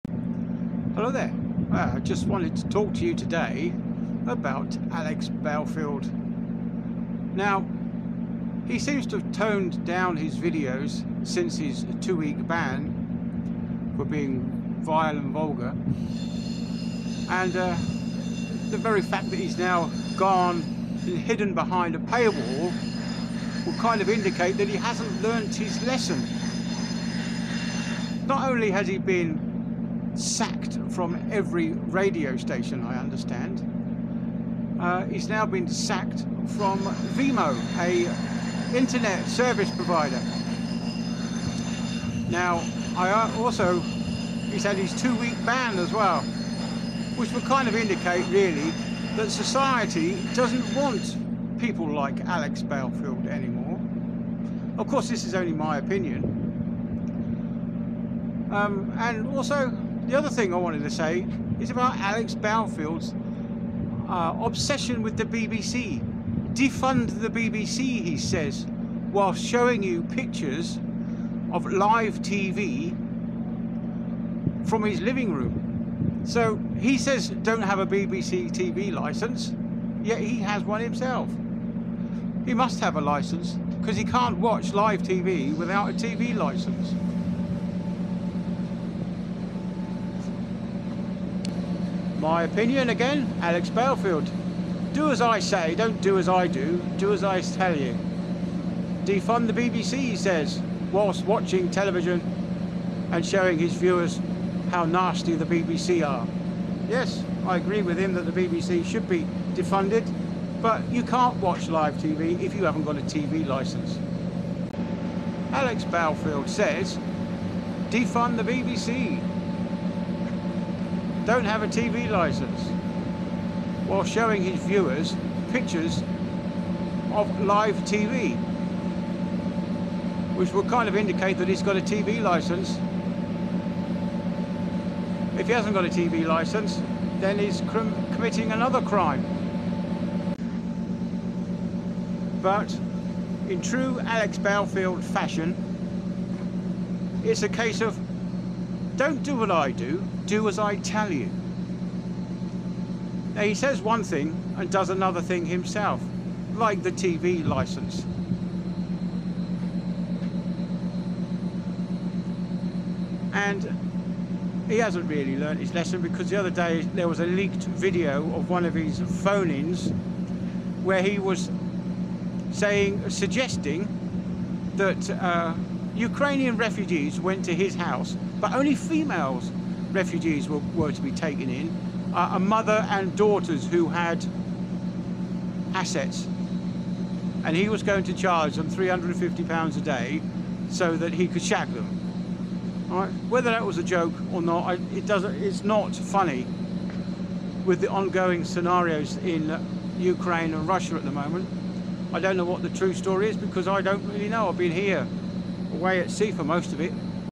Does alex belfield have a tv license
To watch live tv you need a tv license
To you can not record live without a tv license.
Plus one channels are still considered live tv.
Netflix doesnt show live tv so thats safe.
Be careful  with amazon tv as it may show live tv if you load up the wrong apps like iplayer to itv hub.

Or help me fund my camera gear.